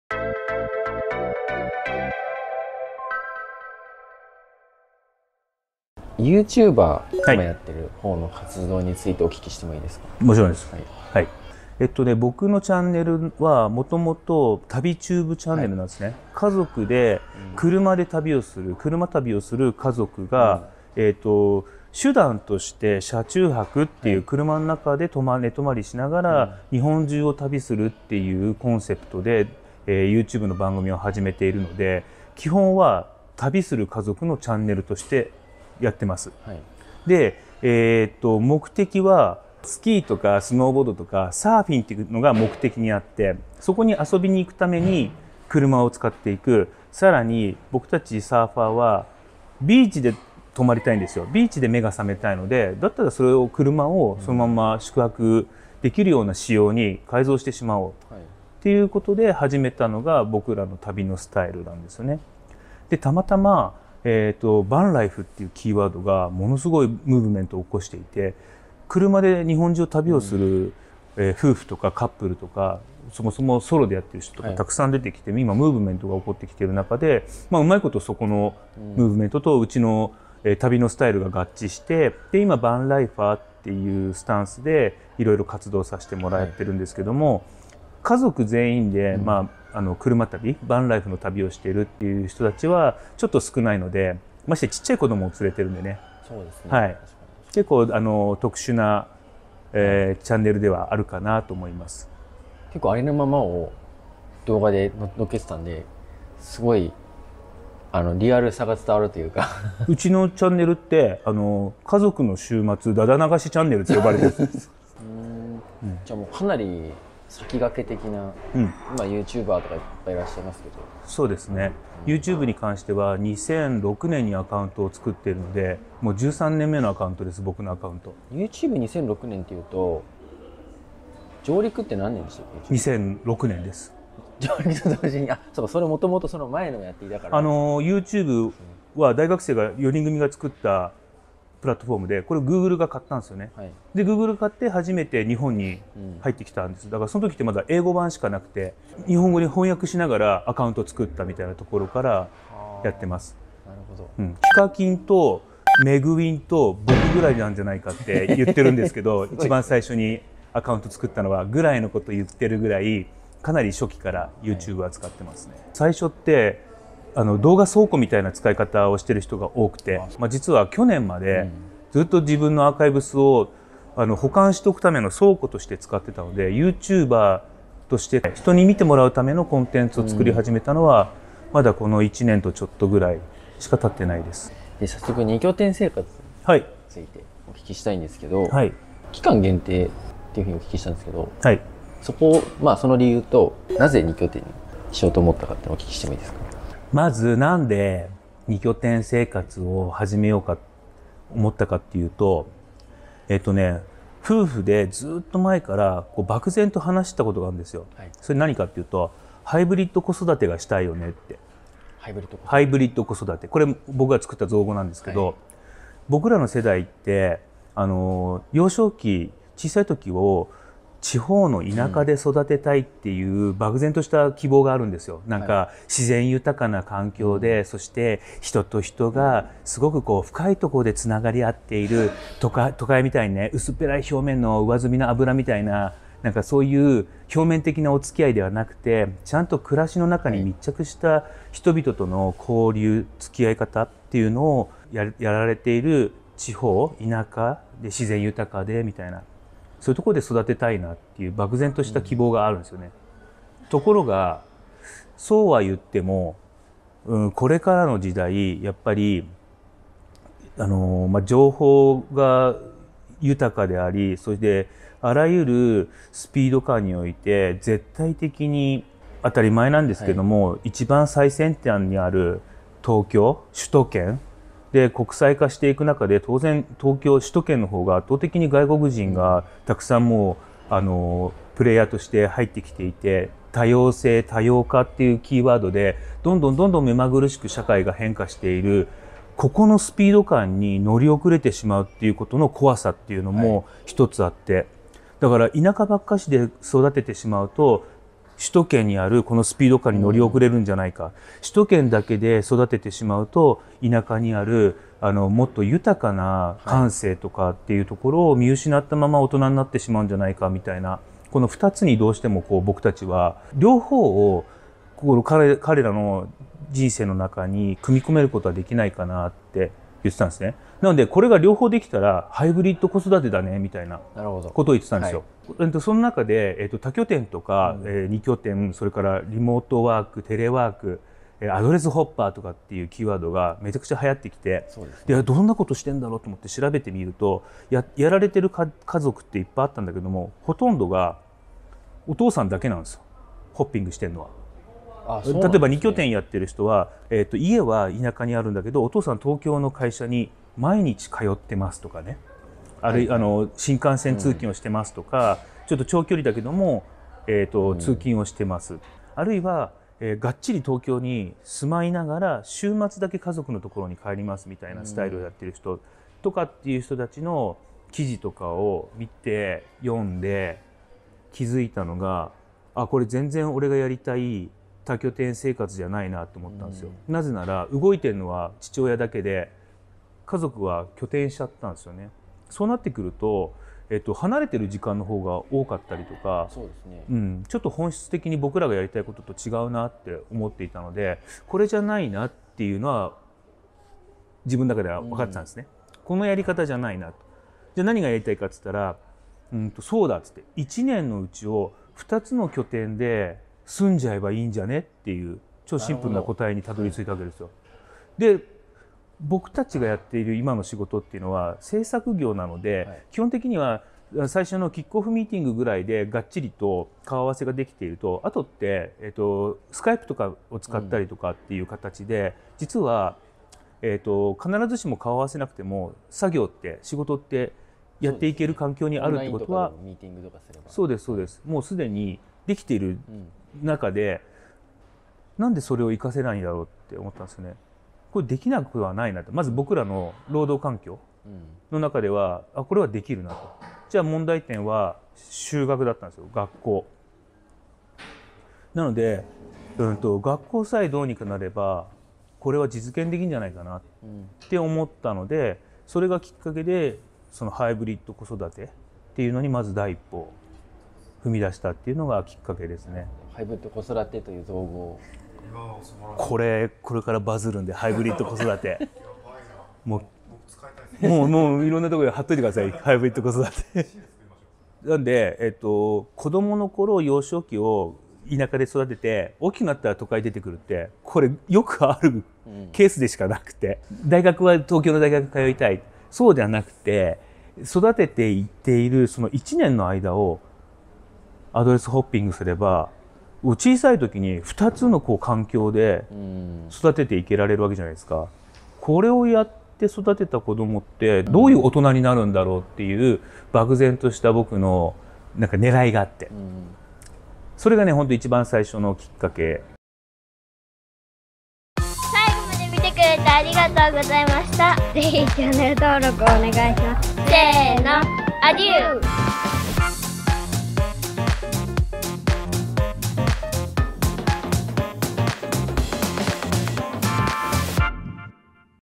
都会で多様性を養いたい。田舎で豊かな感性も養いたい。じゃ両方に住めばいい！【二拠点生活インタビュー#1】
「Going Going Local」のウェブメディアで掲載されているイケさんインタビューの動画です。
まず1回目はYouTuberとしての活動内容紹介と、なぜ家族全員で二拠点生活を始めたのか？その真意を語ります！
全4回の動画です、ぜひ最後までご覧ください。

【Going Going Local】イケさんインタビュー記事

★日本おばかキッズ協会のブログ（とうちゃんがかいちょーをやってます）